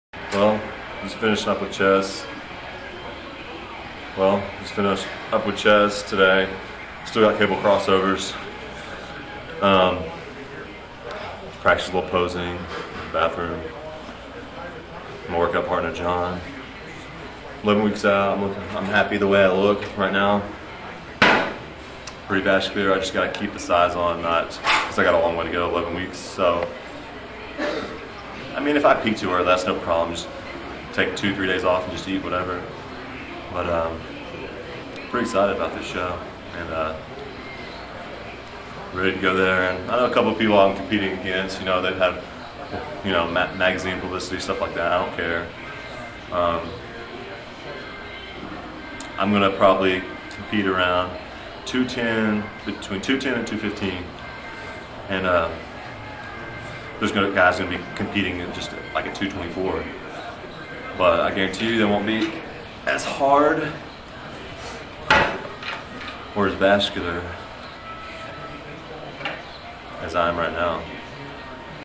Bodybuilder Armon Adibi hits some poses
-- Bodybuilder Armon Adibi goes through some poses as he prepares to compete in the 2007 NPC Junior Nationals in June.